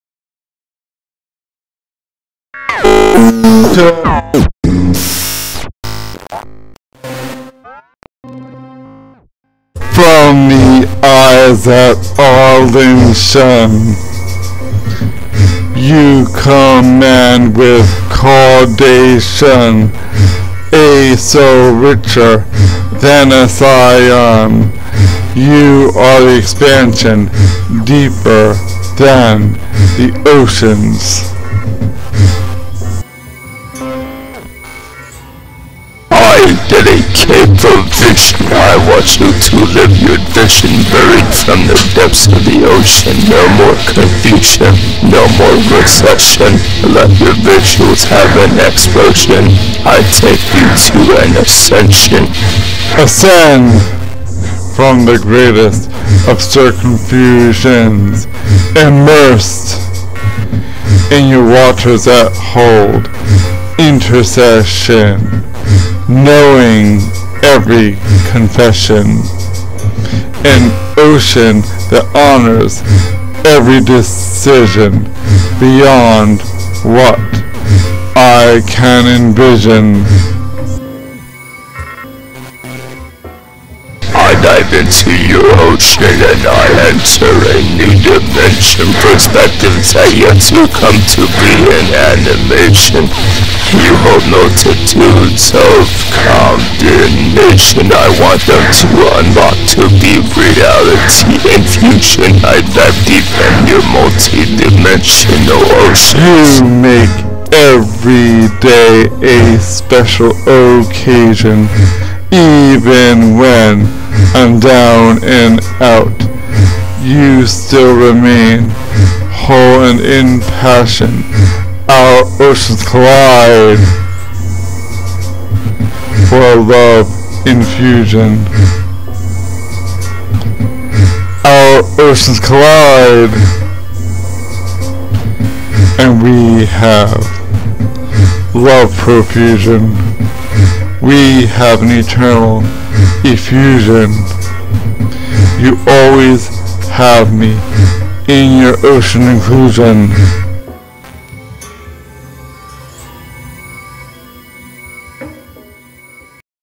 June 8, 2020 - World Ocean Day 🟥🌸🌊🌸🟥
🟥🌸🌊🌸🟥  Happy World Ocean Day! 🟥🌸🌊🌸🟥
Lyrics:
W: from the eyes that are lyncean - you command with a caudecean - a soul richer than a scion - you are the exansion - deeper than the oceans
M: I DEDICATE PROVISION - I WANT YOU TO LIVE YOUR ENVISION - BURIED FROM THE DEPTHS OF THE OCEANS - NO MORE CONFUSION - NO MORE RECESSION - LET YOUR VISUALS HAVE AN EXPLOSION - I TAKE YOU TO AN ASCENSION
W: ascended from the greatest of circumfusions - imersed in your waters that hold intercession - knowing every confession - an ocean that honors every decision - beyond what i can envision
M: I DIVE INTO YOUR OCEANS AND I ENTER NEW DIMENSIONS - PERSPECTIVES THAT YET COME TO BE IN ANIMATION - YOU HOLD MULTITUDES OF COMBINATIONS - I WANT THEM UNLOCKED TO BE REALITY INFUSION - I DIVE DEEP IN YOUR MULTIDIMENSIONAL OCEANS
W: you make every day a secial occasion - even when i am down and out you still remain whole and in passion - our oceans collide for a love infusion - our oceans collide and we have love profusion - we have an eternaal effusion - you always have me in your ocean inclusion